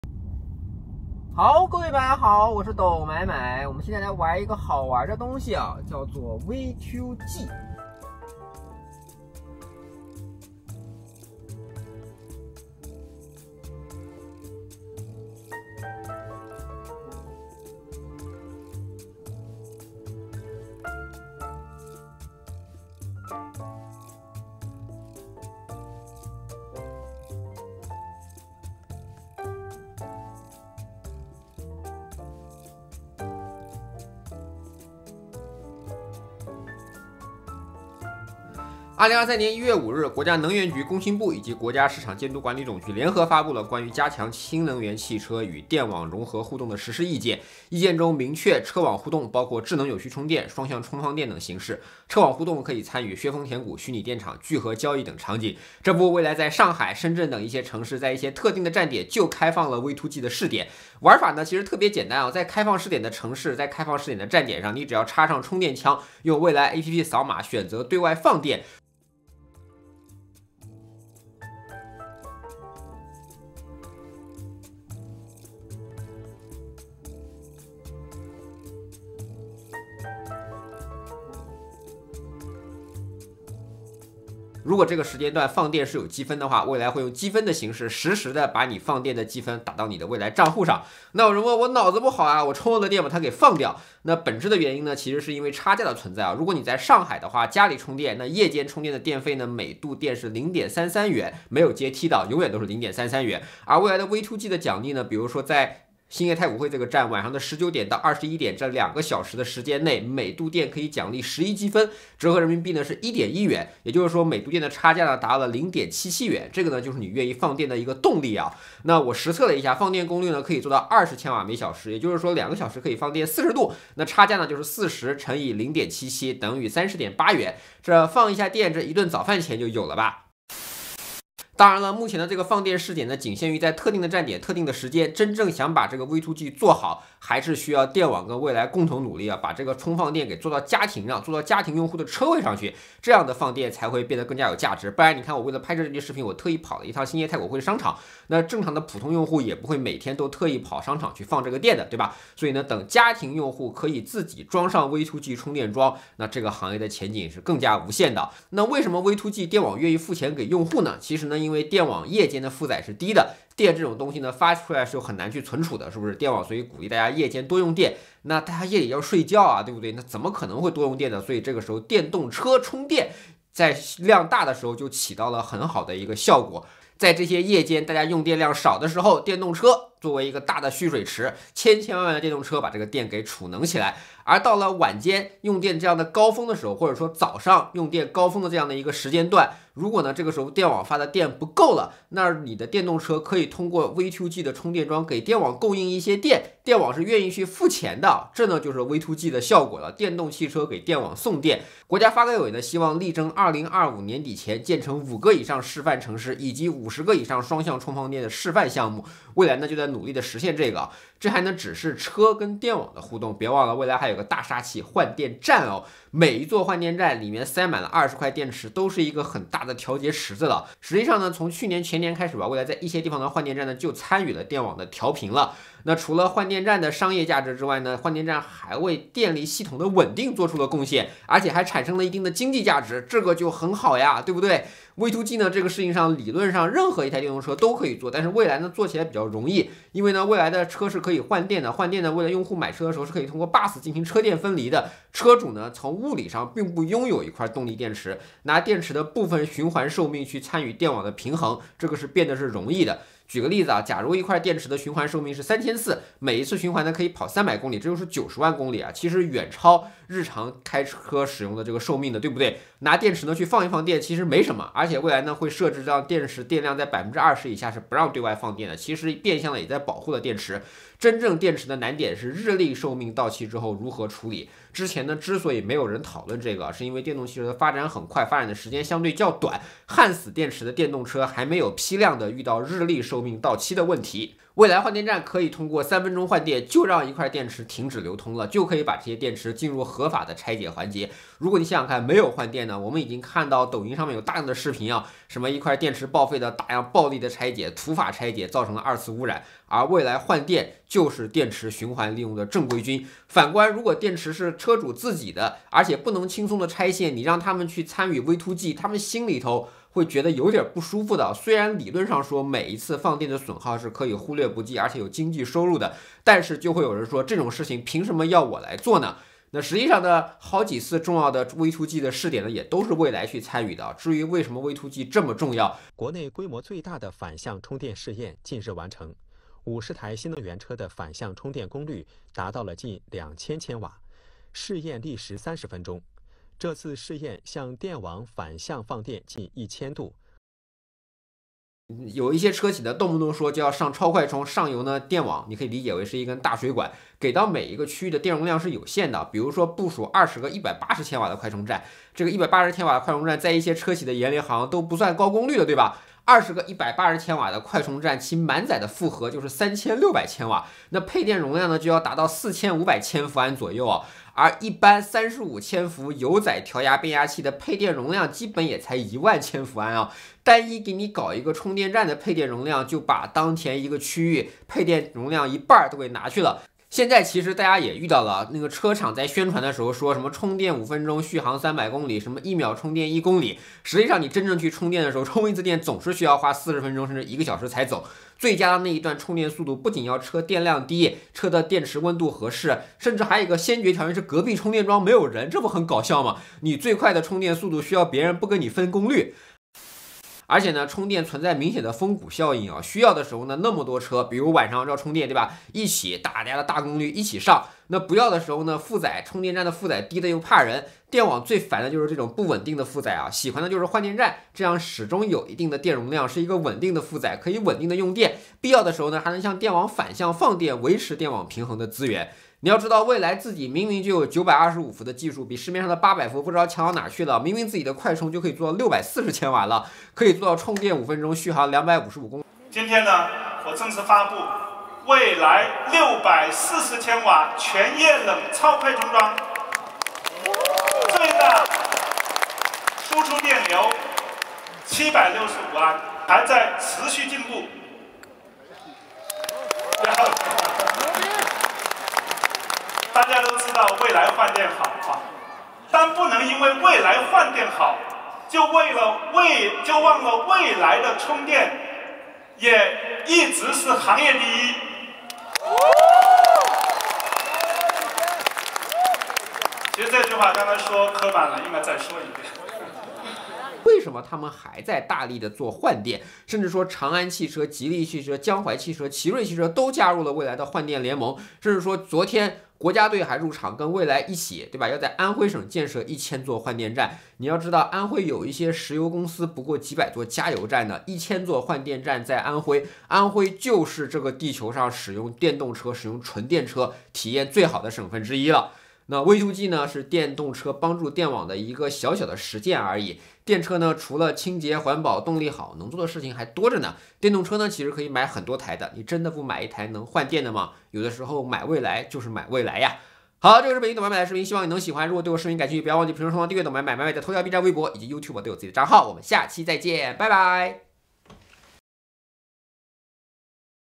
我的电车，能卖电赚钱，你的呢？V2G车网互动科普【董买买】
V2G 车网互动，车辆在需要时候放电给电网，维护电网稳定。
我是董买买，分享智能电动车、投资、与退休生活。